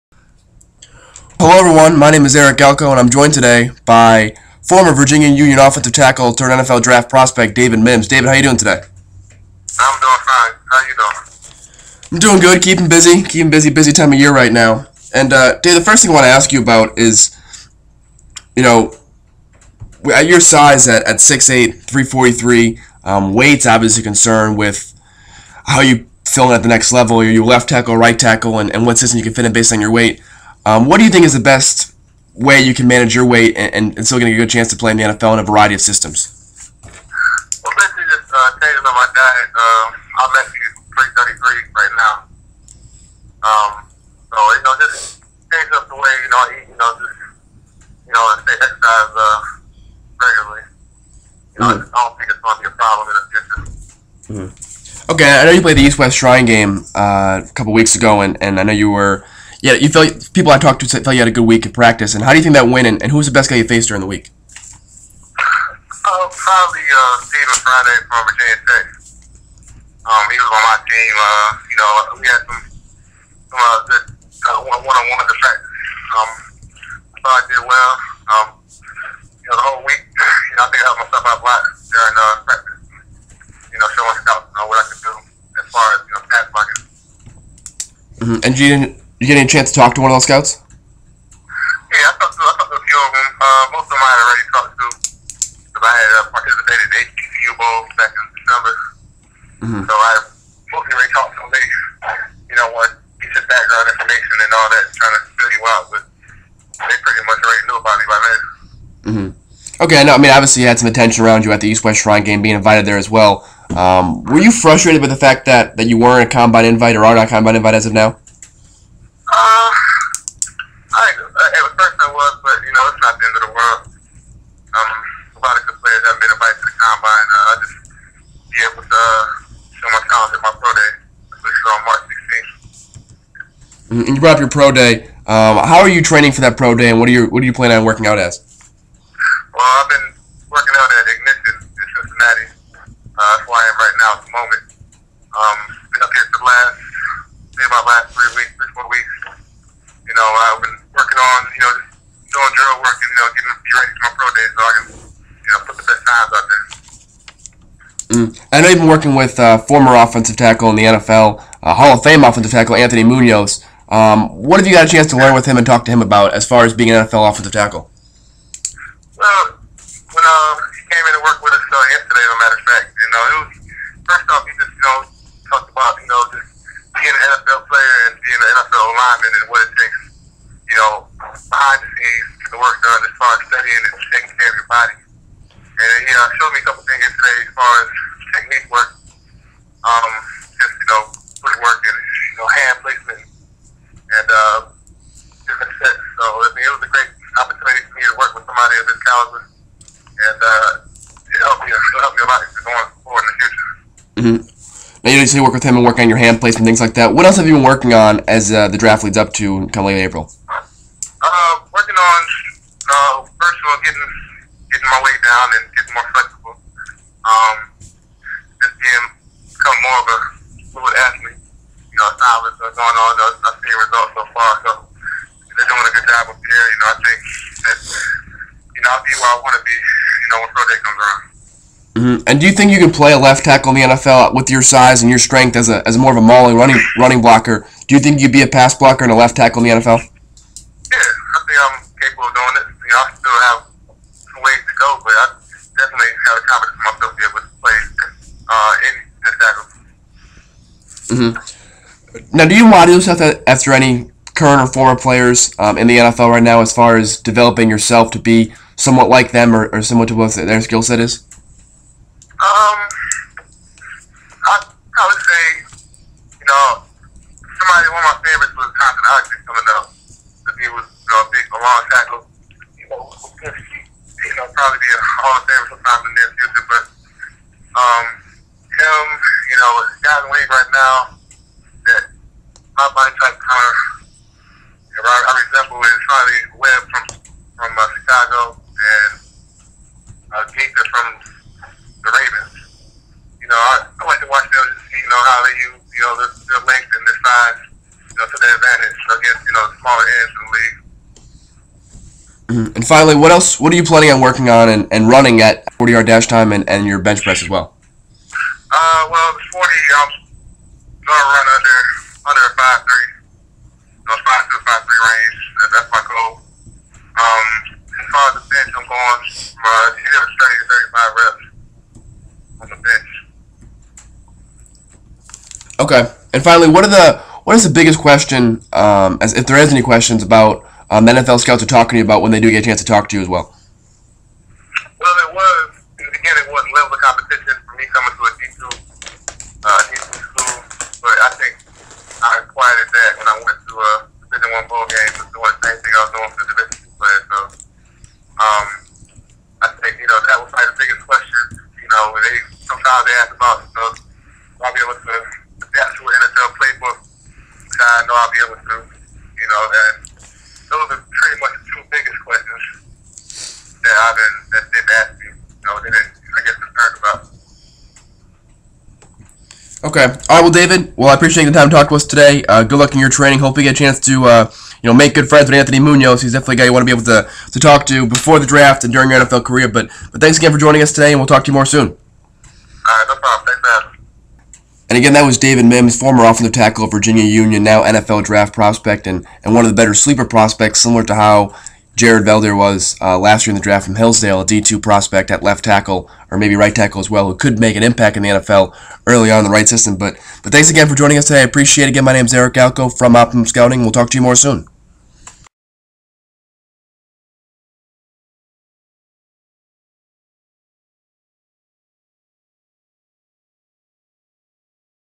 David Mims Interview
An interview with Eric Galko of Optimum Scouting as well as top sleeper offensive tackle prospect David Mims of Virginia Union